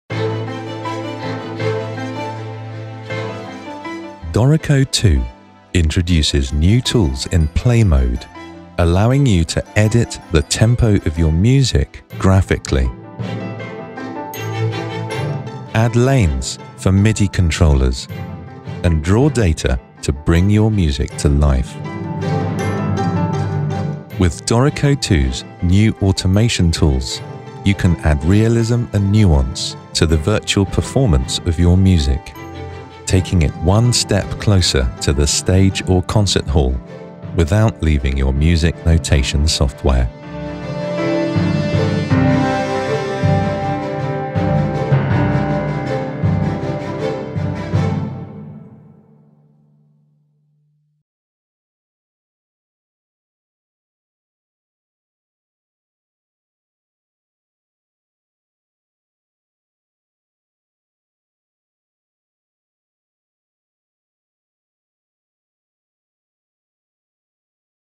Automation in Dorico | New Features in Dorico 2
New tools in Play mode allow you to edit the tempo of your music graphically. Add lanes for MIDI controllers and draw data to bring your music to life. With Dorico 2’s new automation tools you can add realism and nuance to the virtual performance of your music, taking it one step closer to the stage or concert hall without leaving your music notation software.